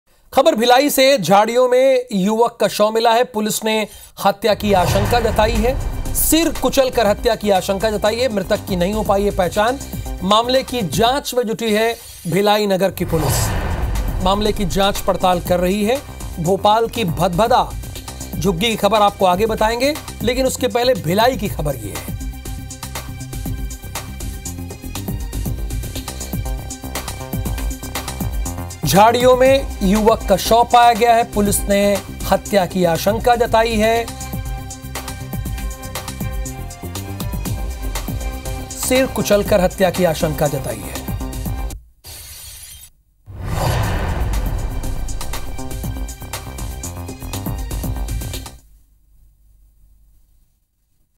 Bhilai News: झाड़ियों में मिला युवक का शव। सिर कुचलकर हत्या की आशंका। देखिए..
IBC24, Bhilai News: झाड़ियों में मिला युवक का शव। सिर कुचलकर हत्या की आशंका। देखिए..